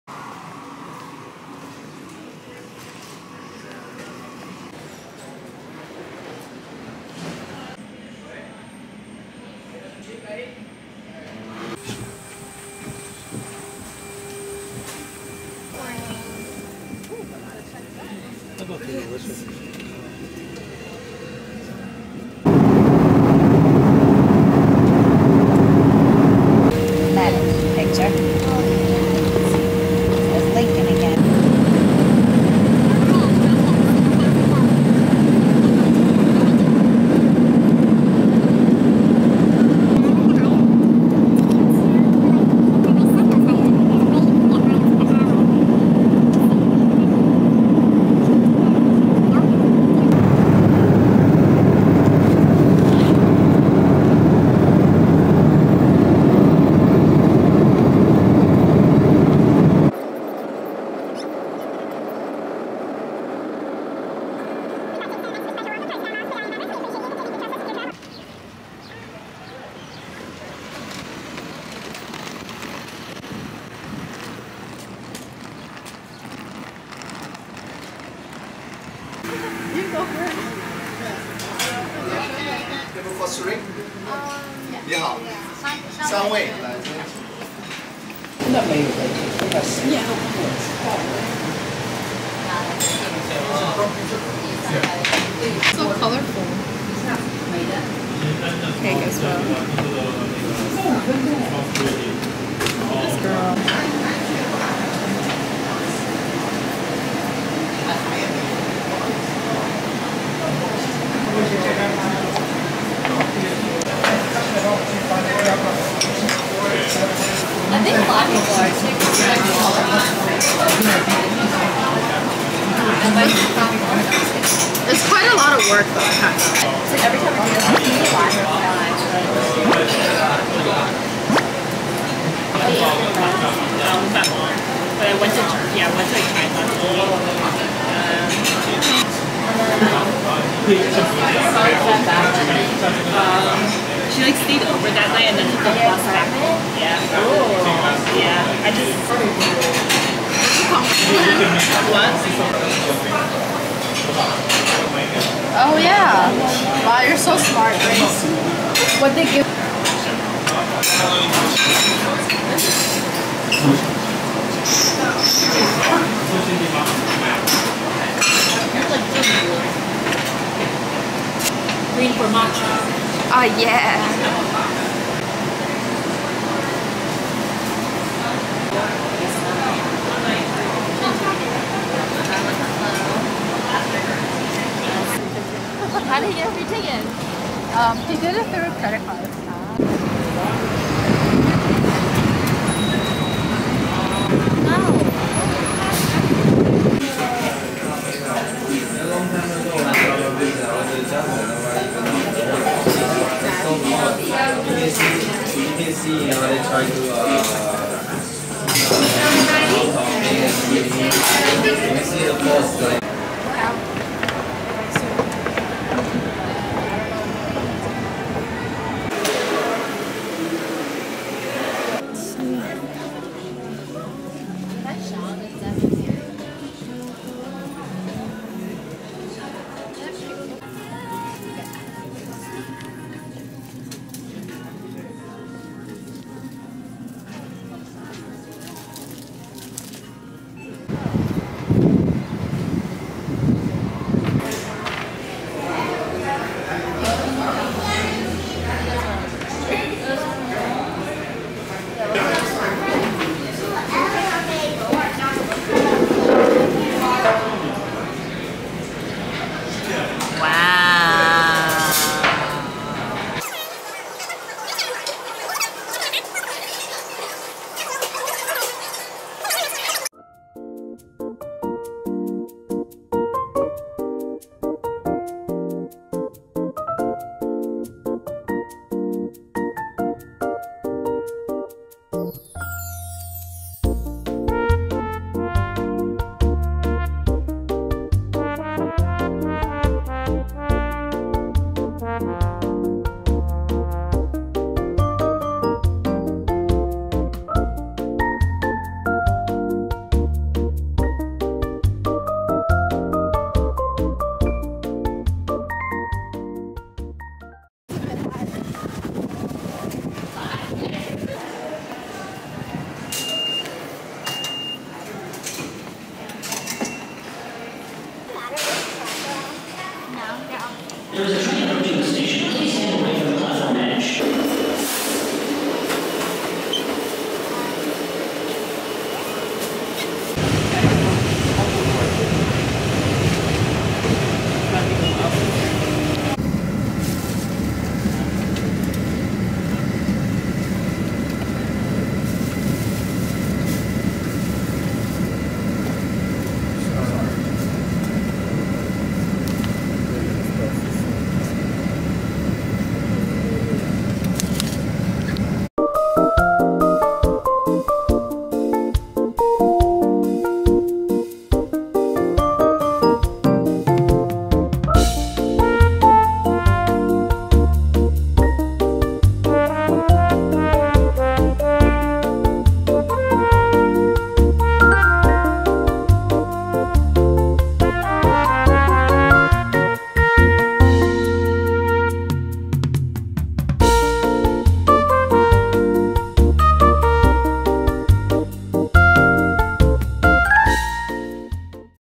Travel Vlog | New York City | XingFu Tang | Plane Ride ✈️
Hello Peeps!!
I am back this time with a travel vlog from my trip to Flushing, New York ☺️
Recently I just graduated highschool and will become a college student in the Fall!
I hope everyone is having a fun summer filled with making wondeful memeories 🥳